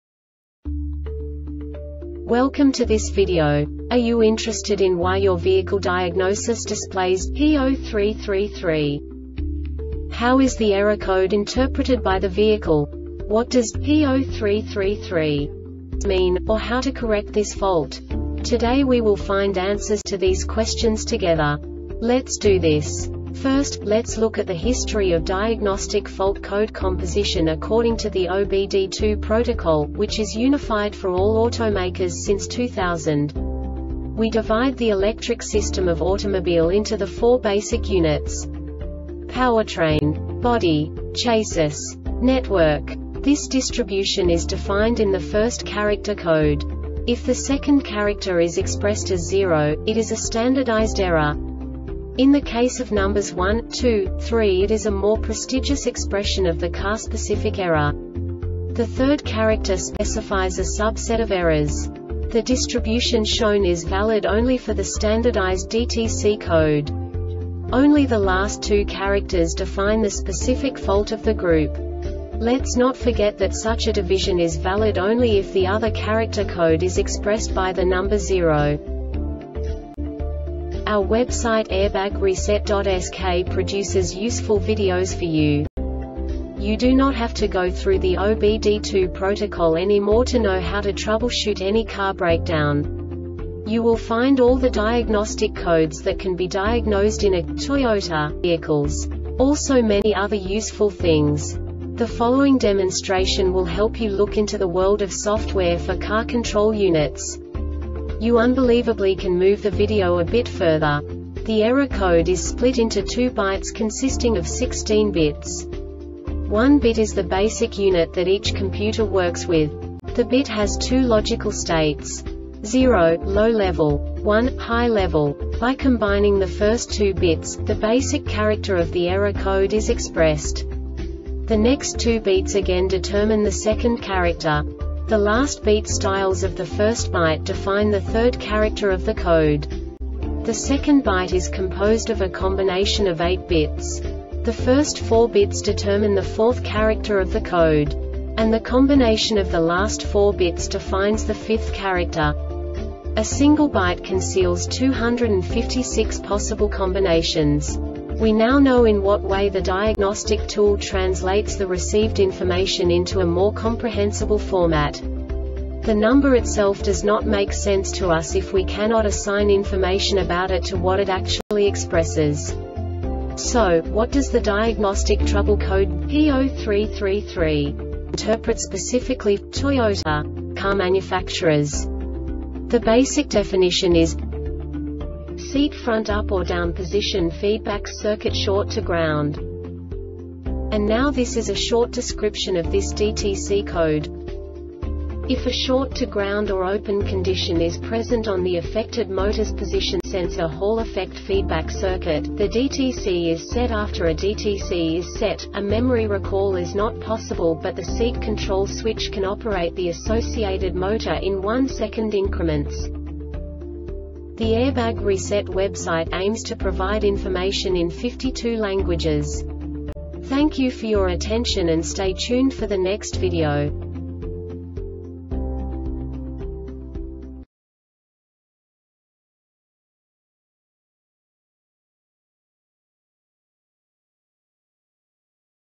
DTC Toyota P0333 Short Explanation
Contents:
0:21 Basic DTC analysis according to OBD2 protocol standard.
1:48 Insight into programming
2:58 A diagnostically understandable description of the Diagnostic Trouble Code
3:05 Basic error definition

Make:
Toyota
Code:
P0333
Definition:
Seat Front Up or Down Position Feedback Circuit Short to Ground
Cause:
If a short to ground or open condition is present on the affected motors position sensor (Hall-effect) feedback circuit,the DTC is set.After a DTC is set,a memory recall is not possible but the seat control switch can operate the associated motor in one-second increments.